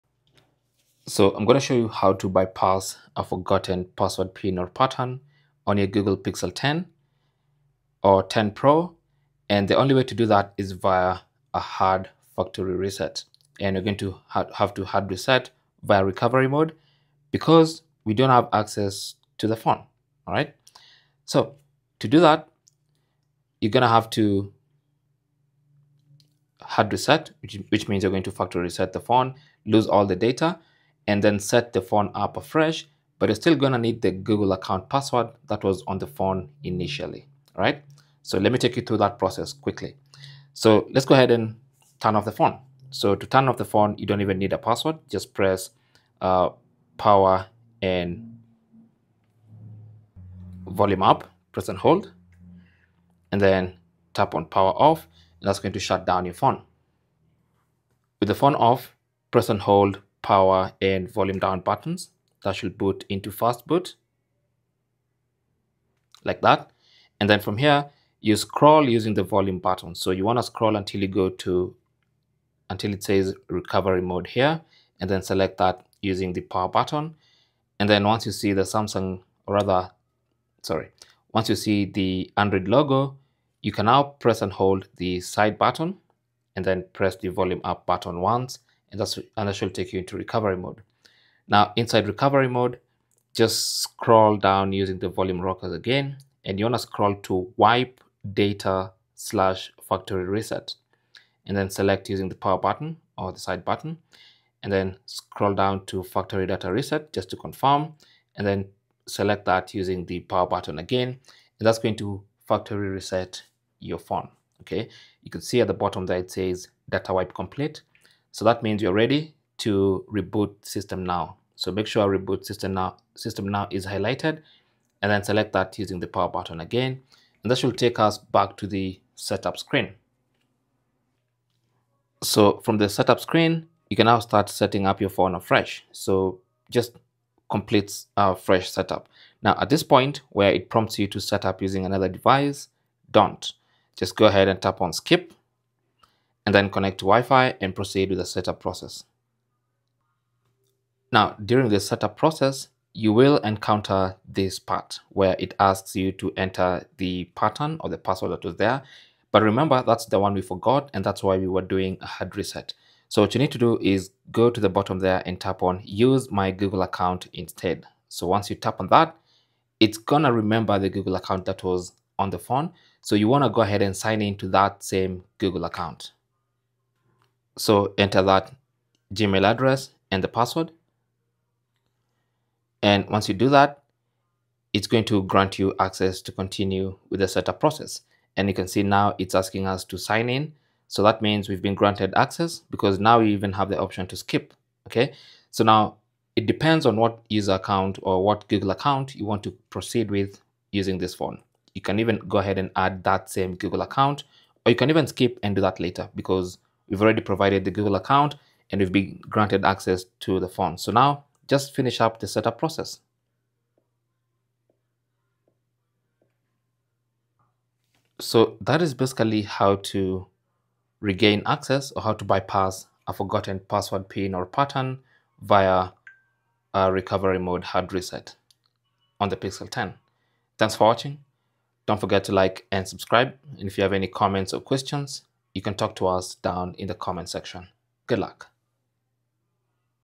Google Pixel 10 Pro: How To Bypass Forgotten Password, PIN or Pattern
Forgot your password, PIN, or pattern on the Google Pixel 10 Pro? Don’t worry—this step-by-step guide will show you how to bypass a locked screen safely and regain access to your device.

Whether you’ve forgotten your PIN, can’t remember your password, or are stuck on a lock screen pattern, this tutorial covers different methods to unlock your Pixel 10 Pro.

Google Pixel 10 / 10 Pro:

Cell Phone Tripod Adapter